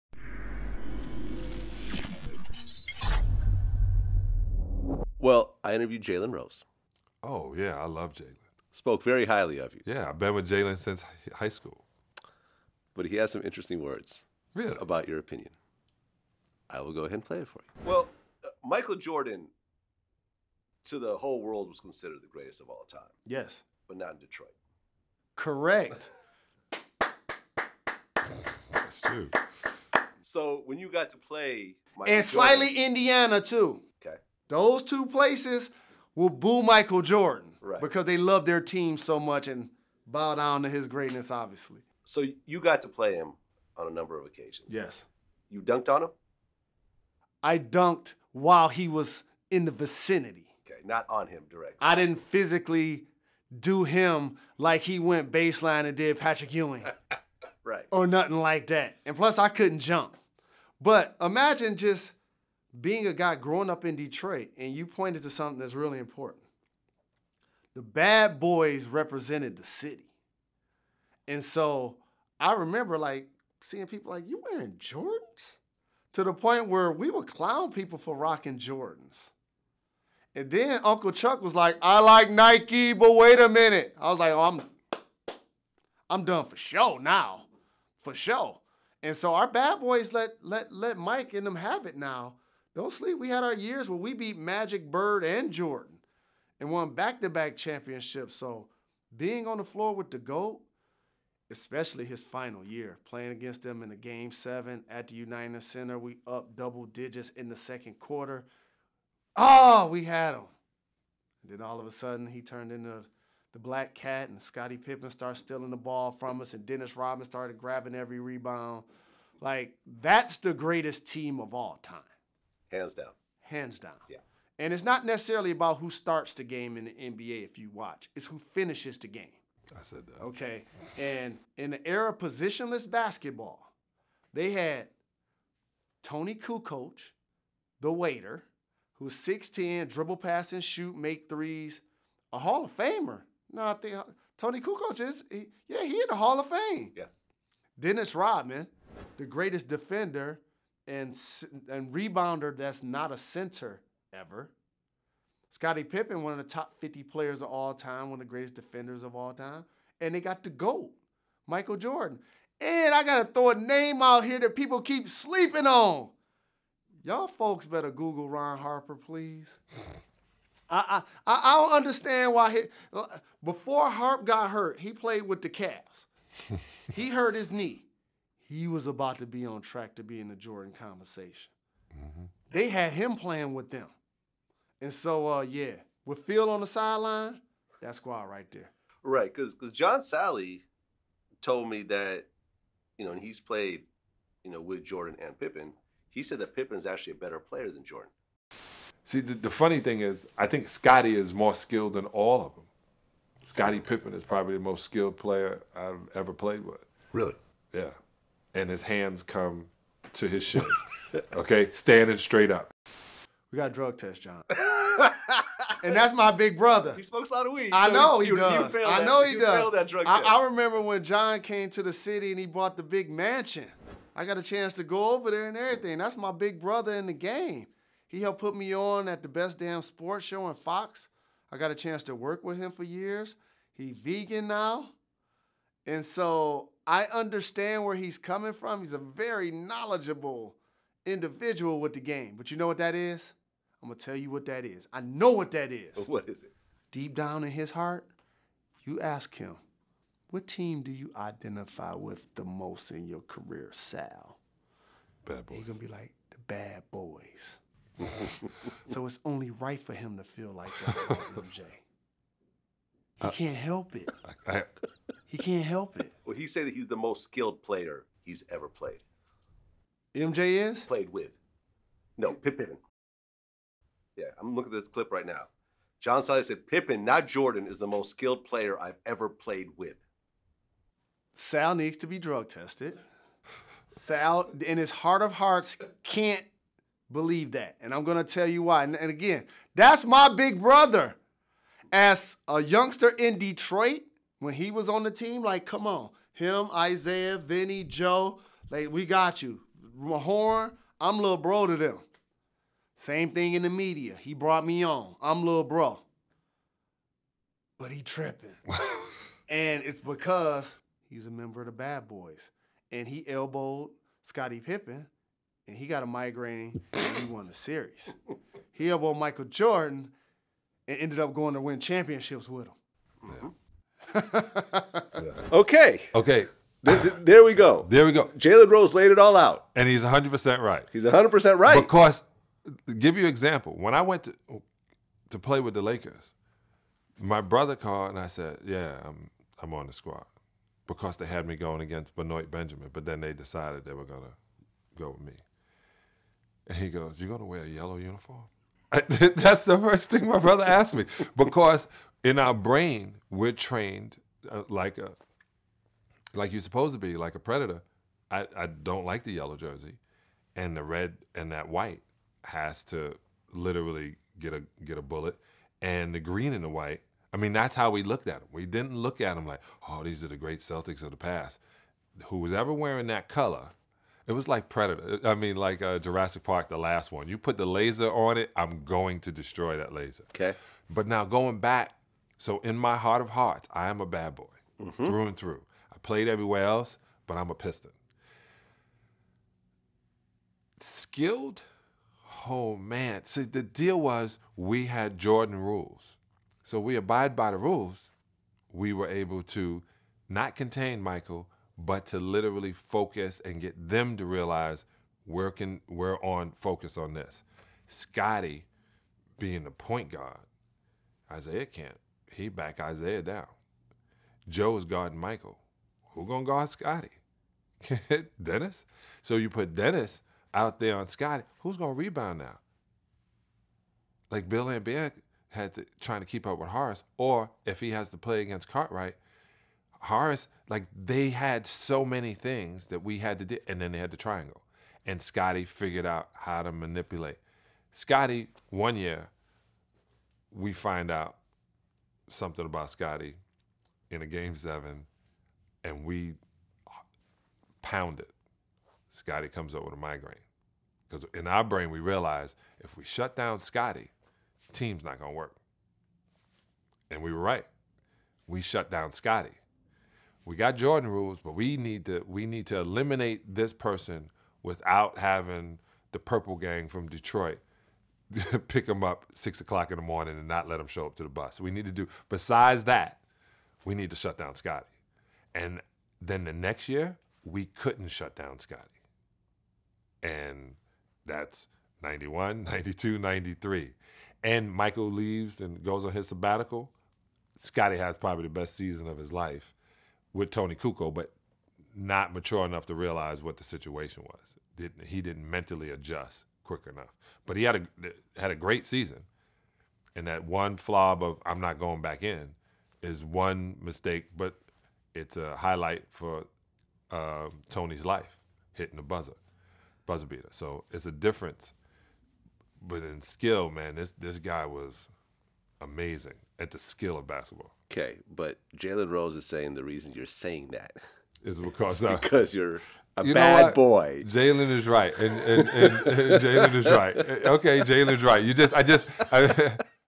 John Salley Agrees with Jalen Rose: I Hate on Jordan Because I'm a "Bad Boy" (Part 2)
John Salley shared his thoughts on Jalen Rose telling VladTV that Salley needed to be drug tested for saying that Scottie Pippen is the most skilled NBA player that he ever played against. Salley explained that Jalen is "100% right" in his assessment that John's role as a member of the Detroit Pistons "Bad Boys" is the reason he can't say Jordan is the most skilled. To hear more of his thoughts, hit the above clip.